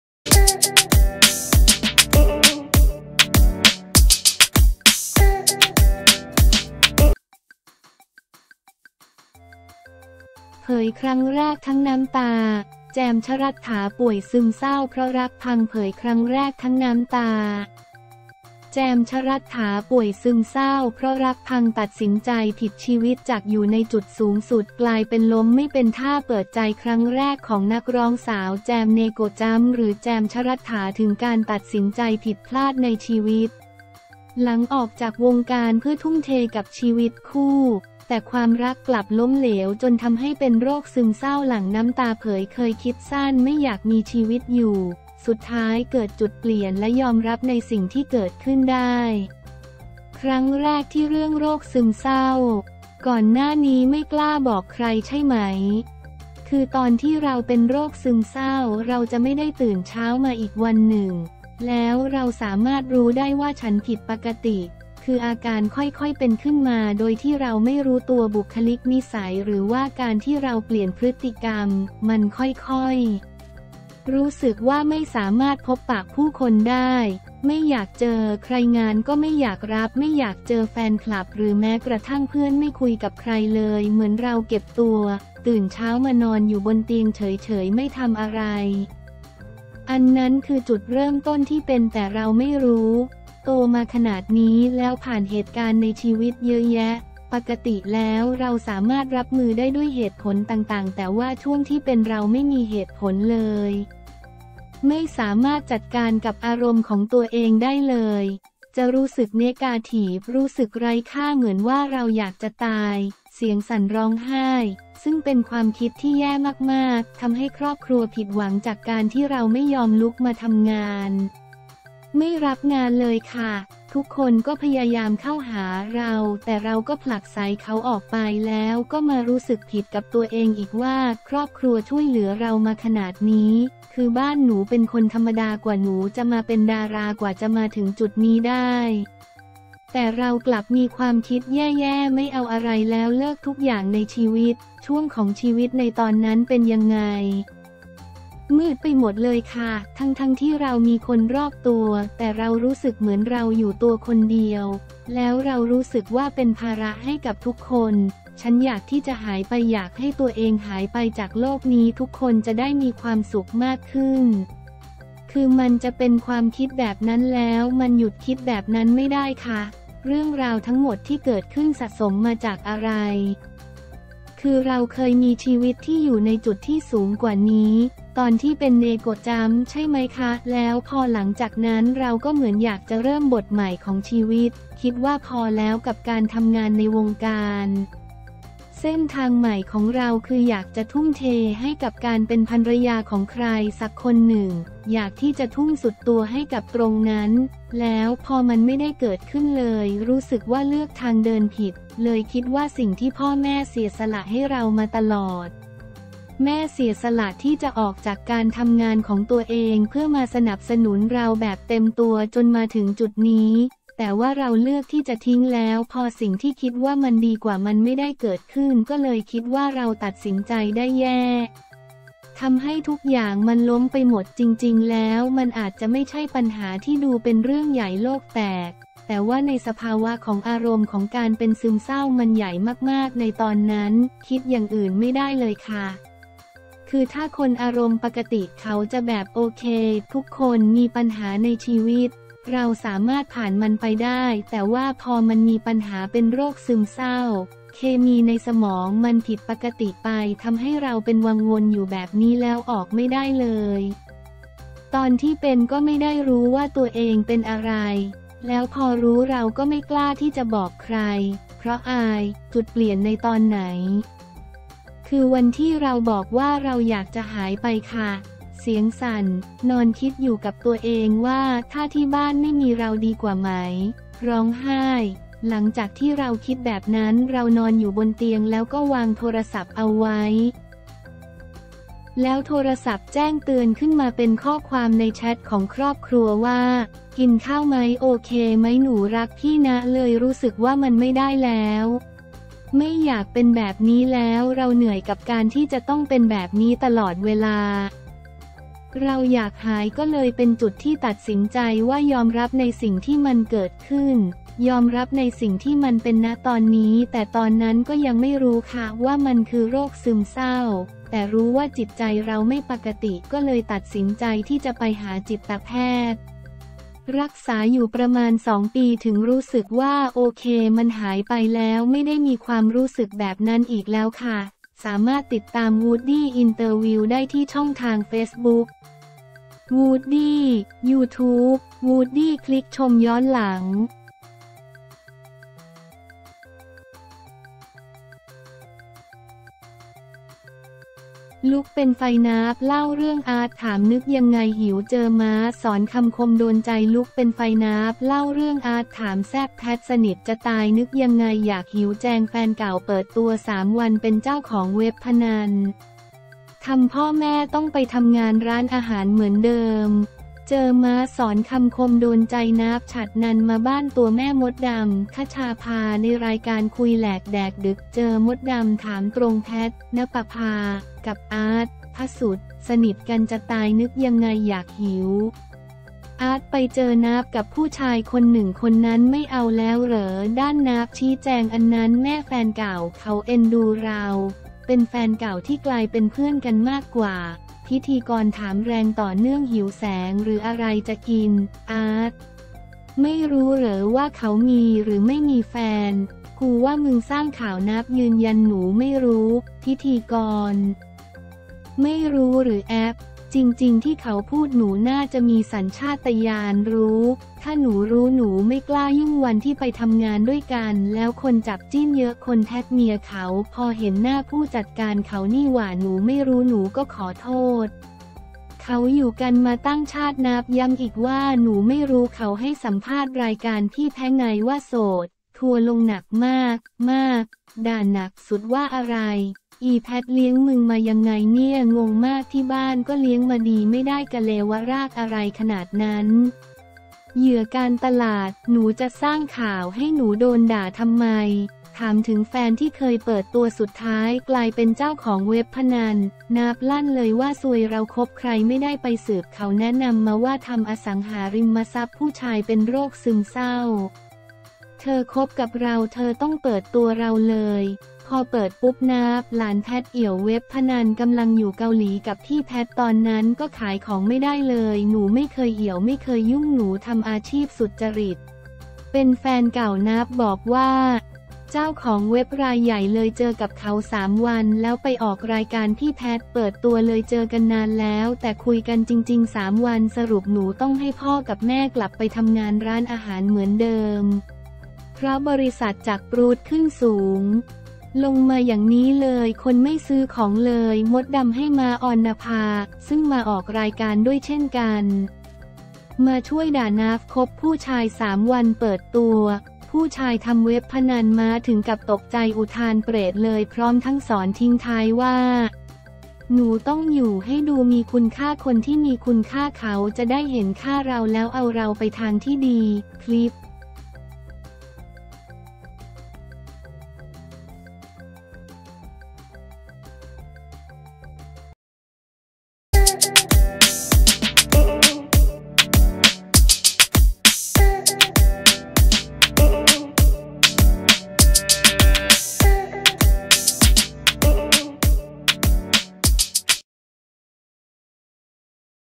เผยครั้งแรกทั้งน้ำตา! แจม ชรัฐฐา ป่วยซึมเศร้า เพราะรักพัง
เผยครั้งแรกทั้งน้ำตา! แจม ชรัฐฐา ป่วยซึมเศร้า เพราะรักพัง ตัดสินใจผิด ชีวิตจากอยู่ในจุดสูงสุด กลายเป็นล้มไม่เป็นท่า
เปิดใจครั้งแรกของนักร้องสาว แจม เนโกะจัมพ์ หรือ แจม ชรัฐฐา ถึงการตัดสินใจผิดพลาดในชีวิต หลังออกจากวงการเพื่อทุ่มเทกับชีวิตคู่ แต่ความรักกลับล้มเหลว จนทำให้เป็นโรคซึมเศร้า หลั่งน้ำตาเผยเคยคิดสั้นไม่อยากมีชีวิตอยู่ สุดท้ายเกิดจุดเปลี่ยนและยอมรับในสิ่งที่เกิดขึ้นได้
ครั้งแรกที่เรื่องโรคซึมเศร้า ก่อนหน้านี้ไม่กล้าบอกใครใช่ไหม? “คือตอนที่เราเป็นโรคซึมเศร้า เราจะไม่ได้ตื่นเช้ามาอีกวันหนึ่ง แล้วเราสามารถรู้ได้ว่าฉันผิดปกติ คืออาการค่อยๆ เป็นขึ้นมาโดยที่เราไม่รู้ตัว บุคลิก นิสัย หรือว่าการที่เราเปลี่ยนพฤติกรรม มันค่อยๆ รู้สึกว่าไม่สามารถพบปะผู้คนได้ ไม่อยากเจอใคร งานก็ไม่อยากรับ ไม่อยากเจอแฟนคลับ หรือแม้กระทั่งเพื่อน ไม่คุยกับใครเลยเหมือนเราเก็บตัว ตื่นเช้ามานอนอยู่บนเตียงเฉยๆ ไม่ทำอะไร”

“อันนั้นคือจุดเริ่มต้นที่เป็นแต่เราไม่รู้ โตมาขนาดนี้แล้วผ่านเหตุการณ์ในชีวิตเยอะแยะ ปกติแล้วเราสามารถรับมือได้ด้วยเหตุผลต่างๆ แต่ว่าช่วงที่เป็นเราไม่มีเหตุผลเลย ไม่สามารถจัดการกับอารมณ์ของตัวเองได้เลย จะรู้สึก ‎Negative รู้สึกไร้ค่า เหมือนว่าเราอยากจะตาย (เสียงสั่น ร้องไห้) ซึ่งเป็นความคิดที่แย่มากๆ ทำให้ครอบครัวผิดหวัง จากการที่เราไม่ยอมลุกมาทำงาน ไม่รับงานเลยค่ะ
“ทุกคนก็พยายามเข้าหาเรา แต่เราก็ผลักไสเขาออกไป แล้วก็มารู้สึกผิดกับตัวเองอีกว่าครอบครัวช่วยเหลือเรามาขนาดนี้ คือบ้านหนูเป็นคนธรรมดา กว่าหนูจะมาเป็นดารา กว่าจะมาถึงจุดนี้ได้ แต่เรากลับมีความคิดแย่ๆ ไม่เอาอะไรแล้ว เลิกทุกอย่างในชีวิต”

ช่วงของชีวิตในตอนนั้นเป็นยังไง? “มืดไปหมดเลยค่ะ ทั้งๆ ที่เรามีคนรอบตัว แต่เรารู้สึกเหมือนเราอยู่ตัวคนเดียว แล้วเรารู้สึกว่าเป็นภาระให้กับทุกคน ฉันอยากที่จะหายไป อยากให้ตัวเองหายไปจากโลกนี้ทุกคนจะได้มีความสุขมากขึ้น คือมันจะเป็นความคิดแบบนั้น แล้วมันหยุดคิดแบบนั้นไม่ได้ค่ะ
เรื่องราวทั้งหมดที่เกิดขึ้นสะสมมาจากอะไร? “คือเราเคยมีชีวิตที่อยู่ในจุดที่สูงกว่านี้ ตอนที่เป็น เนโกะจัมพ์ ใช่มั้ยคะ แล้วพอหลังจากนั้นเราก็เหมือนอยากจะเริ่มบทใหม่ของชีวิต คิดว่าพอแล้วกับการทำงานในวงการ เส้นทางใหม่ของเราคืออยากจะทุ่มเทให้กับการเป็นภรรยาของใครสักคนหนึ่ง อยากที่จะทุ่มสุดตัวให้กับตรงนั้น แล้วพอมันไม่ได้เกิดขึ้น เลยรู้สึกว่าเลือกทางเดินผิด”

“เลยคิดว่าสิ่งที่พ่อแม่เสียสละให้เรามาตลอด แม่เสียสละที่จะออกจากการทำงานของตัวเองเพื่อมาสนับสนุนเราแบบเต็มตัวจนมาถึงจุดนี้ แต่ว่าเราเลือกที่จะทิ้ง แล้วพอสิ่งที่คิดว่ามันดีกว่า มันไม่ได้เกิดขึ้นก็เลยคิดว่าเราตัดสินใจได้แย่ ทำให้ทุกอย่างมันล้มไปหมด จริงๆ แล้วมันอาจจะไม่ใช่ปัญหาที่ดูเป็นเรื่องใหญ่โลกแตก แต่ว่าในสภาวะของอารมณ์ของการเป็นซึมเศร้ามันใหญ่มากๆ ในตอนนั้น
“คิดอย่างอื่นไม่ได้เลยค่ะ คือถ้าคนอารมณ์ปกติเขาจะแบบโอเคทุกคนมีปัญหาในชีวิต เราสามารถผ่านมันไปได้ แต่ว่าพอมันมีปัญหาเป็นโรคซึมเศร้า เคมีในสมองมันผิดปกติไป ทำให้เราเป็นวังวนอยู่แบบนี้แล้วออกไม่ได้เลย ตอนที่เป็นก็ไม่ได้รู้ว่าตัวเองเป็นอะไร แล้วพอรู้เราก็ไม่กล้าที่จะบอกใคร เพราะอาย”

จุดเปลี่ยนในตอนไหน? “คือวันที่เราบอกว่า เราอยากจะหายไปค่ะ (เสียงสั่น) นอนคิดอยู่กับตัวเองว่าถ้าที่บ้านไม่มีเราดีกว่าไหม (ร้องไห้) หลังจากที่เราคิดแบบนั้น เรานอนอยู่บนเตียงแล้วก็วางโทรศัพท์เอาไว้ แล้วโทรศัพท์แจ้งเตือนขึ้นมาเป็นข้อความในแชทของครอบครัวว่า กินข้าวมั้ย โอเคมั้ย หนูรักพี่นะ เลยรู้สึกว่ามันไม่ได้แล้ว ไม่อยากเป็นแบบนี้แล้ว เราเหนื่อยกับการที่จะต้องเป็นแบบนี้ตลอดเวลา
“เราอยากหายก็เลยเป็นจุดที่ตัดสินใจว่ายอมรับในสิ่งที่มันเกิดขึ้น ยอมรับในสิ่งที่มันเป็น ณ ตอนนี้ แต่ตอนนั้นก็ยังไม่รู้ค่ะว่ามันคือโรคซึมเศร้า แต่รู้ว่าจิตใจเราไม่ปกติ ก็เลยตัดสินใจที่จะไปหาจิตแพทย์ รักษาอยู่ประมาณ 2 ปี ถึงรู้สึกว่าโอเคมันหายไปแล้ว ไม่ได้มีความรู้สึกแบบนั้นอีกแล้วค่ะ”

สามารถติดตาม WOODY INTERVIEW ได้ที่ช่องทาง Facebook: Woody , Youtube: Woody